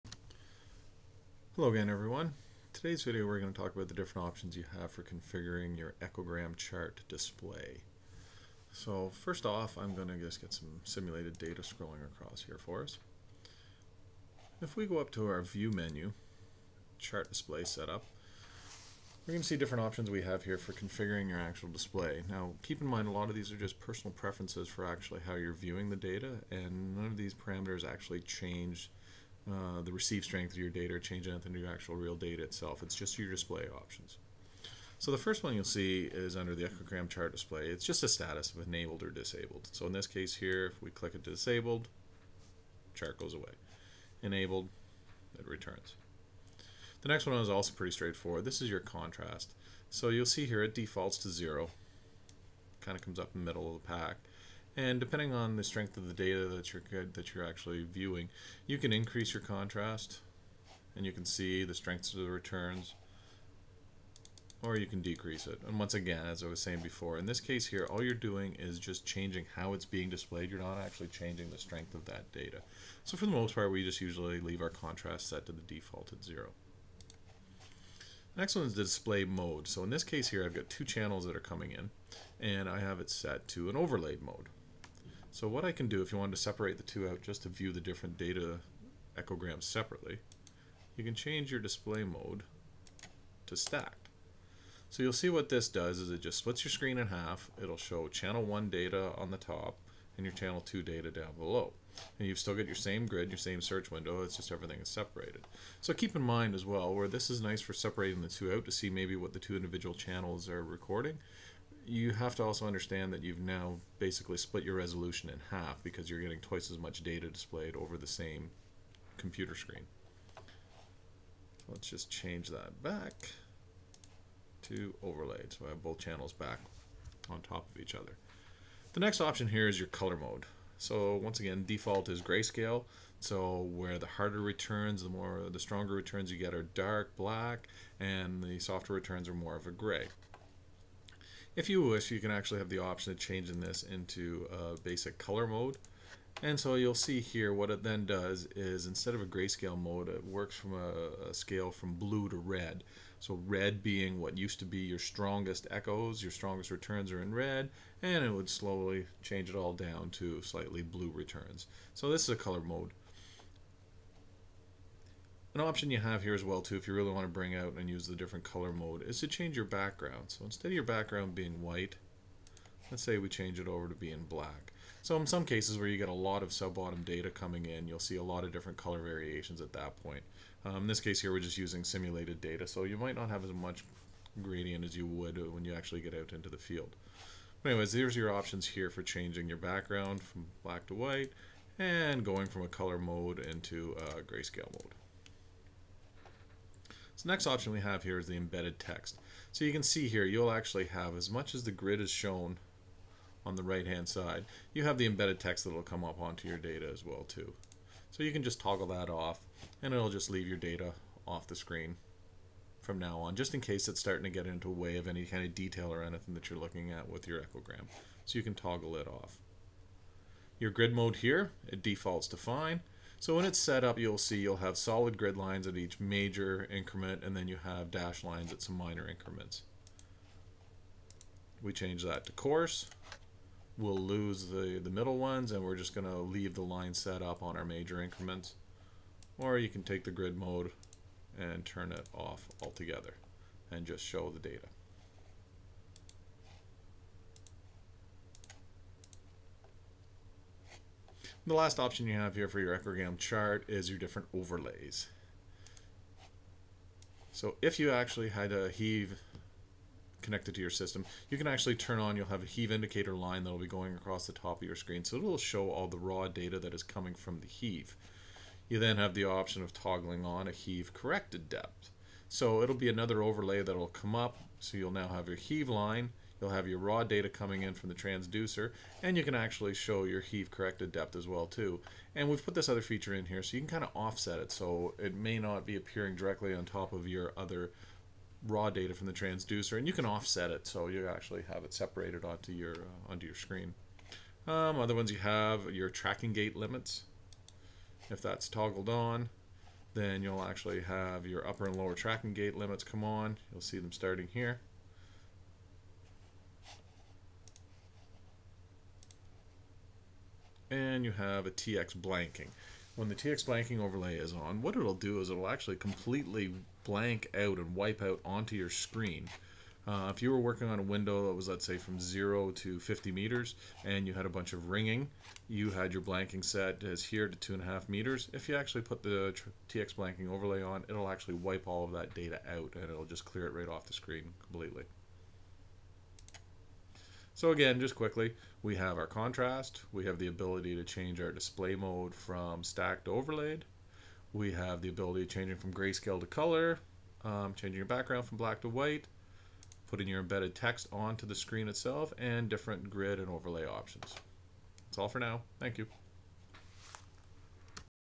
Support Video 7: Chart Display Options
Review of the controls impacting the chart display presentation